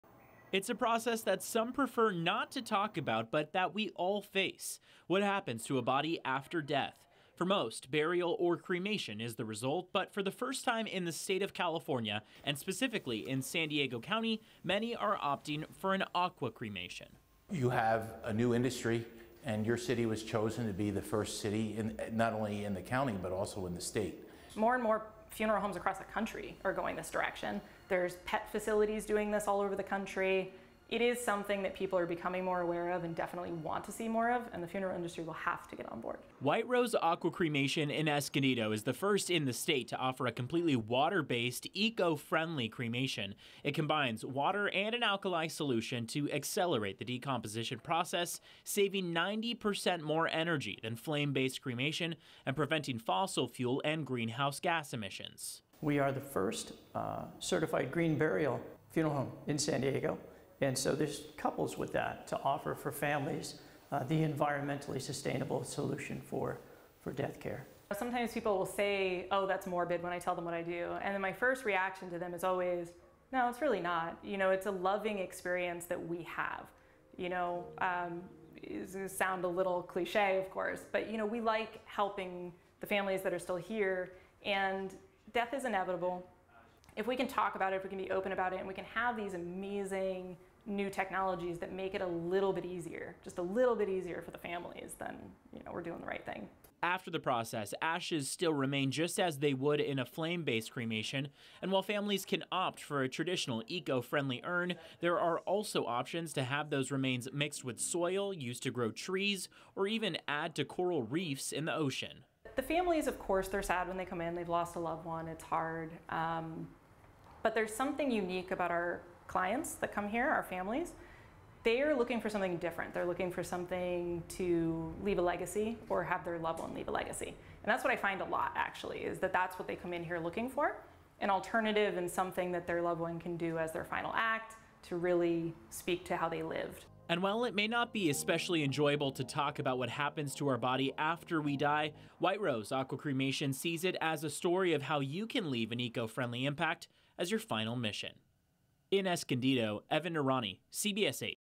First California-based water cremation facility opens
The first flameless cremation facility for humans in the state of California is now open in San Diego County.

White Rose Aqua Cremation in Escondido offers a completely water-based, eco-friendly end of life. It combines water and an alkali solution to accelerate the decomposition process – saving 90 percent more energy than flame-based cremation and preventing fossil fuel and greenhouse gas emissions.

“You have a new industry and your city was chosen to be the first person not only in the county, but also in the state,” said Escondido Mayor, Paul McNamara.

It’s a process that some prefer not to talk about, but that we all face – what happens to a body after death. For most, burial or flame-based cremation is the result.